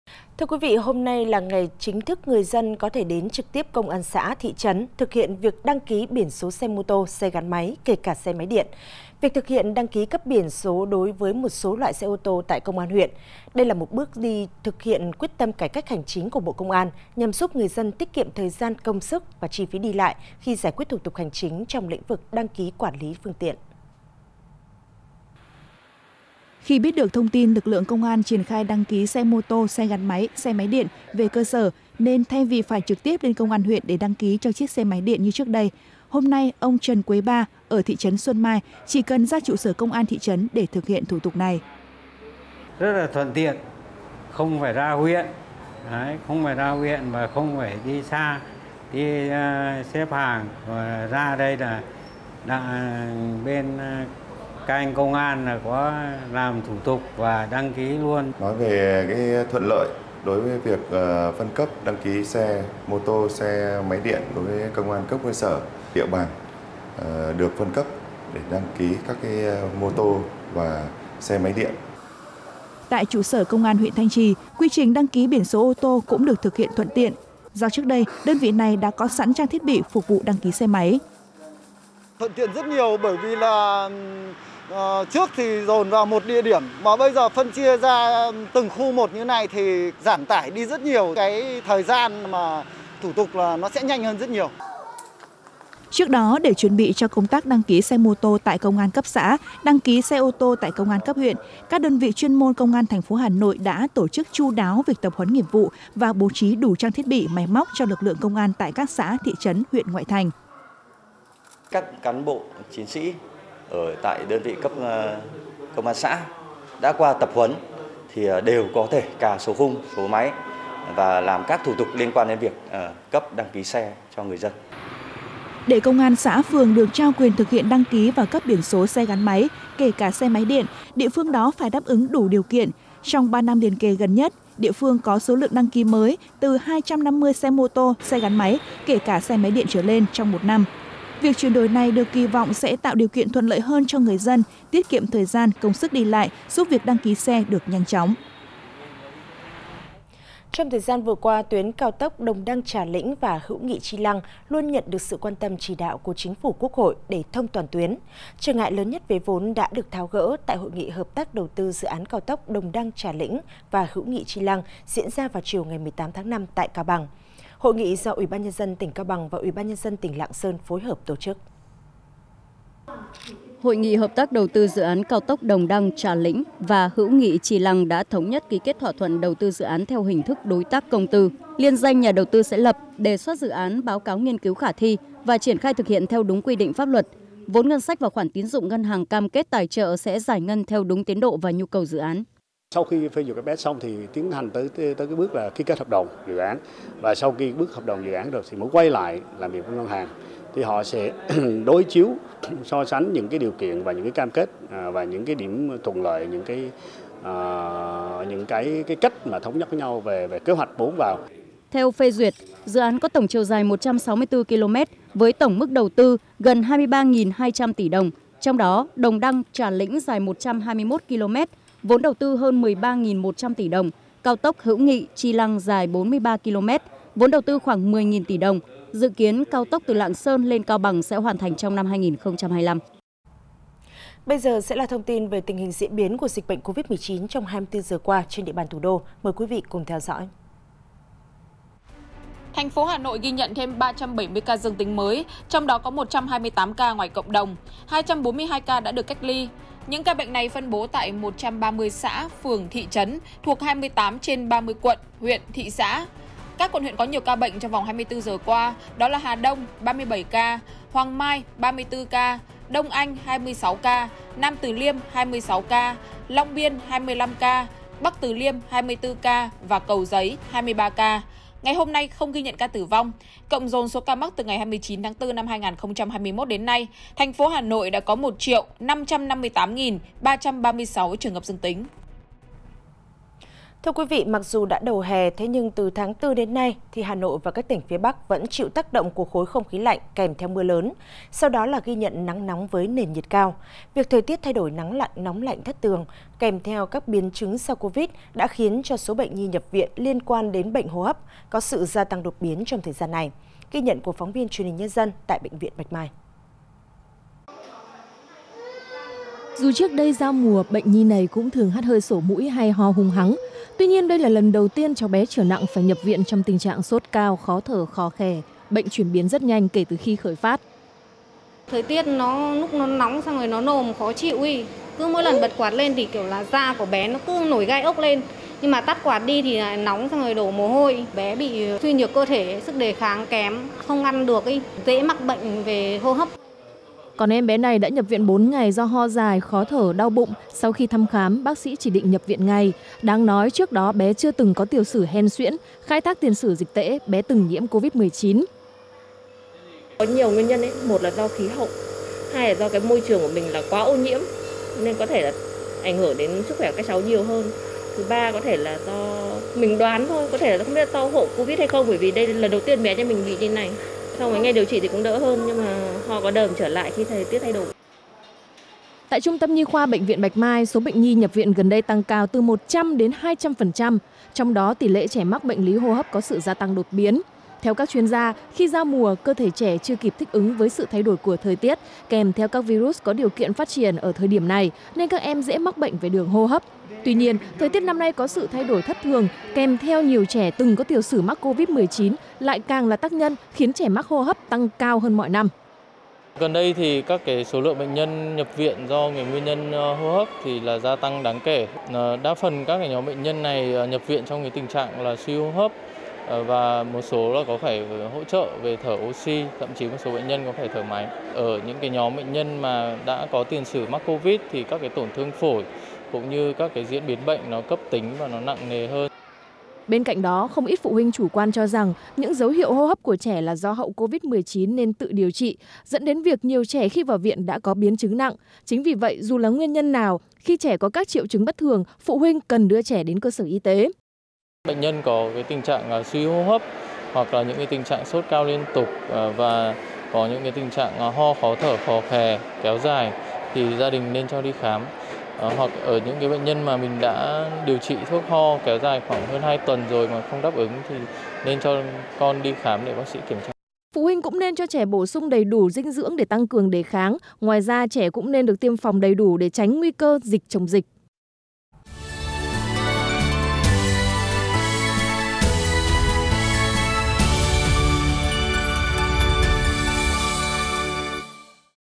Chính thức đăng ký ô tô, xe máy tại Công an xã, huyện | Tin đời sống, kinh tế xã hội cuối ngày 21/5
Chính thức đăng ký ô tô, xe máy tại Công an xã, huyện | Tin đời sống, kinh tế xã hội cuối ngày 21/5 | Truyền Hình Nhân Dân
1. Dự Báo Thời Tiết Hôm Nay